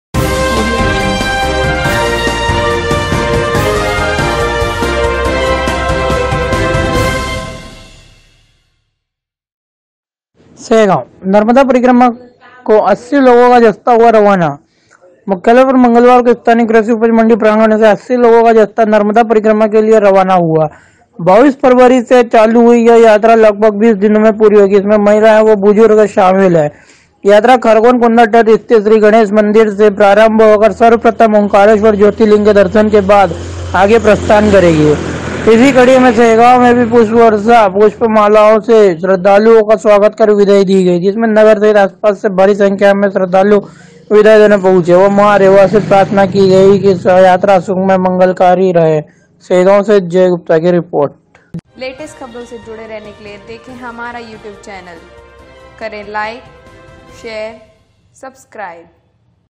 अस्त्र न्यूज़ सेगांव : नर्मदा परिक्रमा को 80 लोगो का जस्ता हुआ रवाना
सेगांव

नर्मदा परिक्रमा को 80 लोगो का जस्ता हुआ रवाना

मुख्यालय पर मंगलवार को स्थानीय कृषि उपज मंडी प्रांगण से 80 लोगों का जत्था नर्मदा परिक्रमा के लिए रवाना हुआ। 22 फरवरी से चालू हुई यह यात्रा लगभग 20 दिनों में पूरी होगी। इसमें महिलाएं व बुजुर्ग शामिल है। यात्रा खरगोन कुंदा तट स्थित श्री गणेश मंदिर से प्रारंभ होकर सर्वप्रथम ओम्कारेश्वर ज्योतिर्लिंग के दर्शन के बाद आगे प्रस्थान करेगी। इसी कड़ी में सेगांव मैं भी पुष्प मालाओं से श्रद्धालुओं को स्वागत कर विदाई दी गई। जिसमें नगर सहित आसपास के भारी संख्या में श्रद्धालु विदाई देने पहुंचे। व मां रेवा से प्रार्थना की गई की यात्रा सुखमय मंगलकारी रहे।

सेगांव से जय गुप्ता की रिपोर्ट